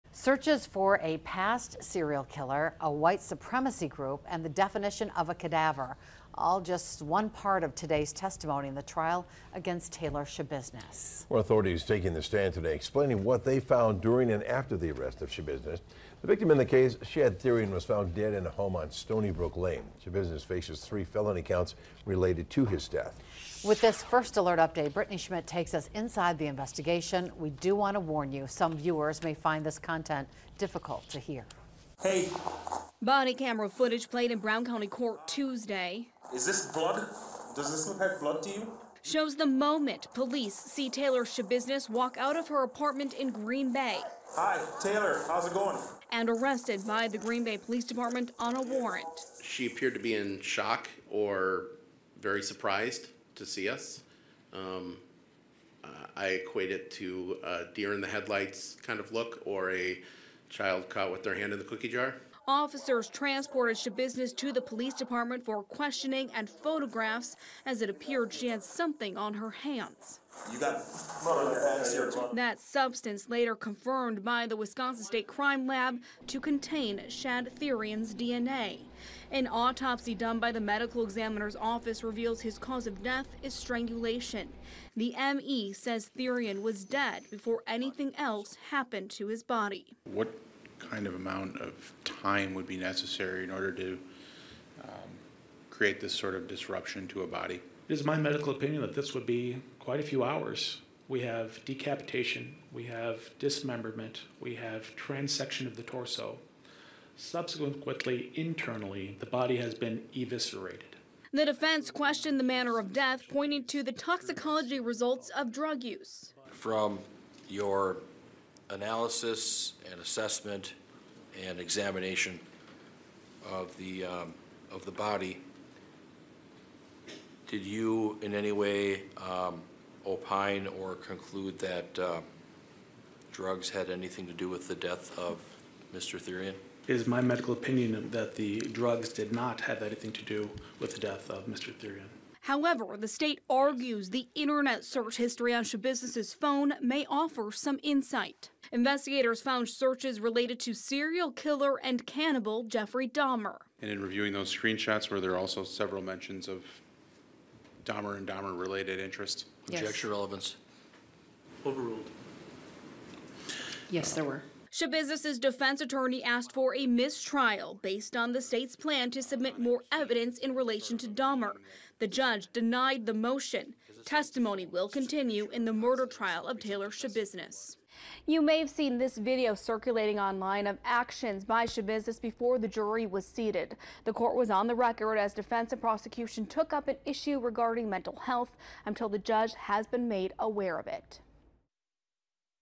Taylor Schabusiness trial: Internet search history includes Jeffrey Dahmer
On Tuesday, the state referred to Internet search history on Schabusiness’s phone.